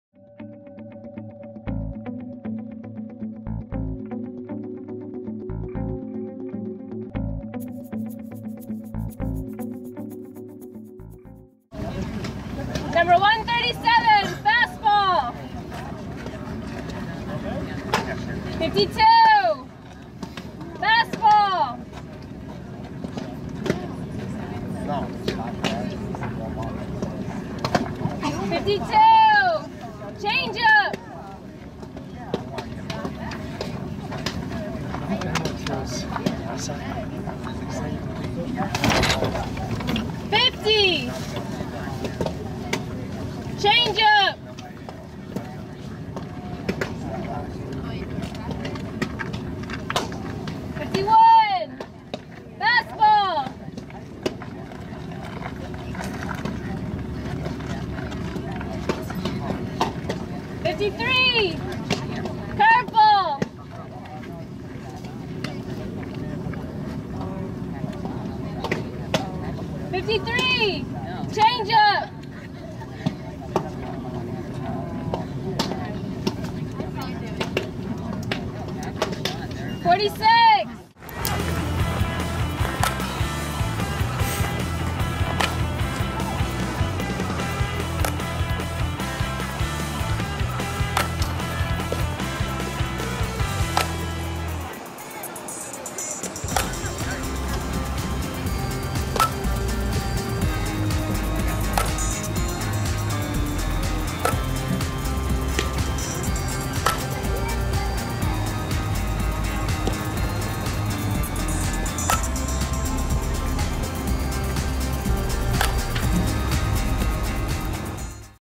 137 Megan Beebe Class of 2019
Filmed at the USA Elite Northern Exposure Recruiting Camp July 13, 2017